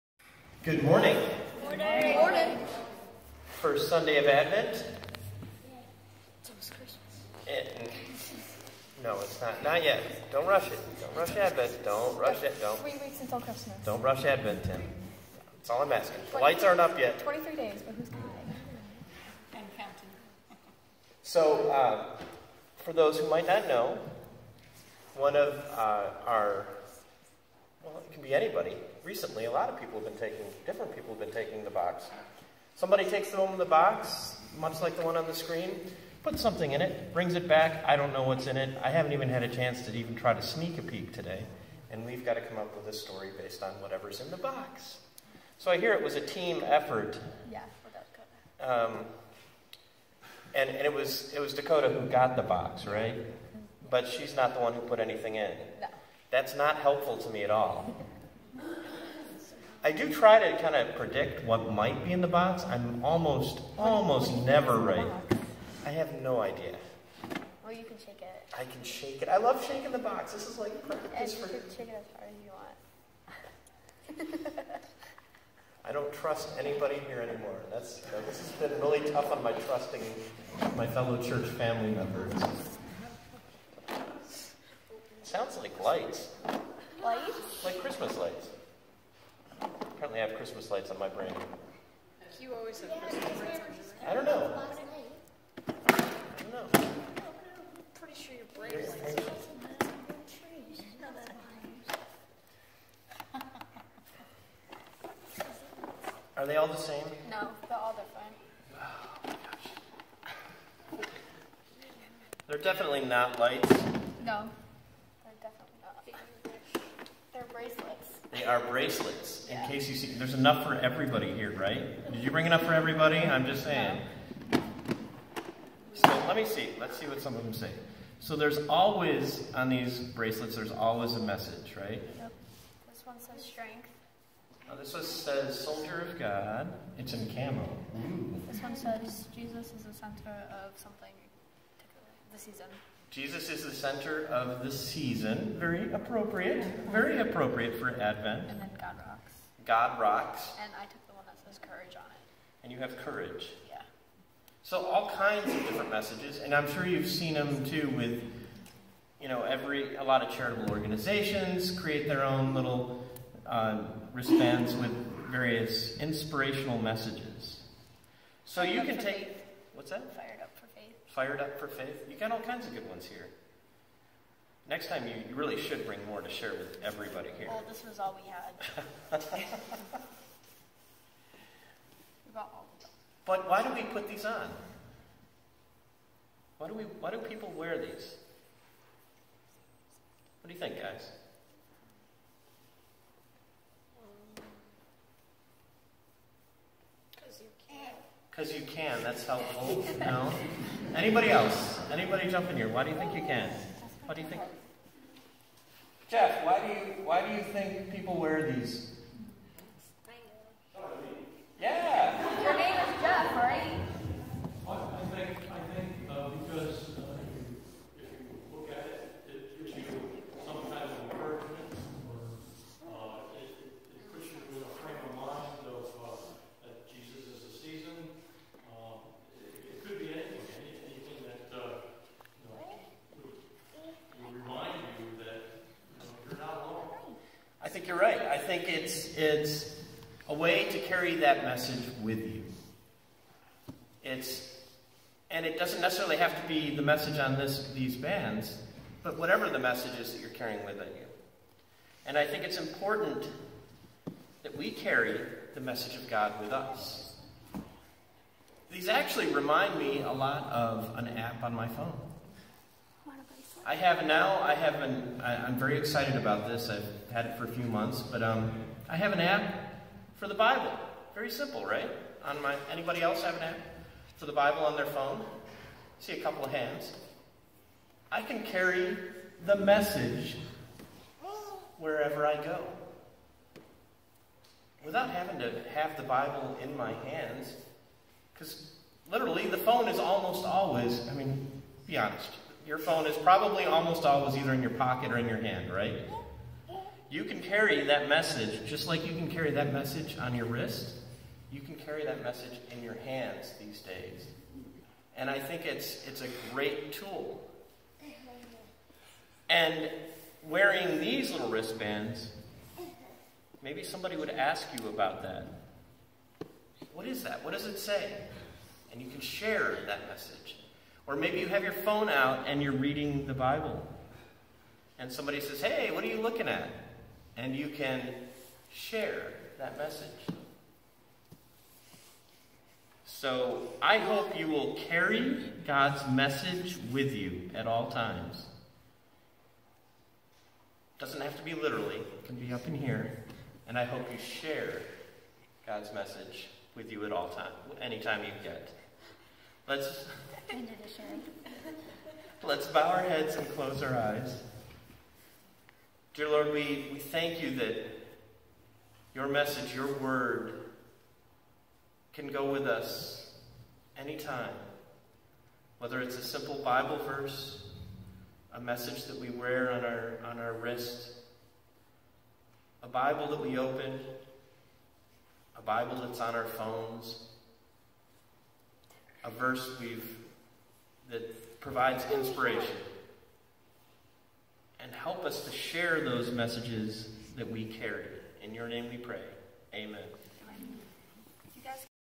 Wristband
Wristbands help us show support for the causes we love. When we wear them it reminds us of what we support and may prompt others to ask us about it. What if we could do the same with the bible?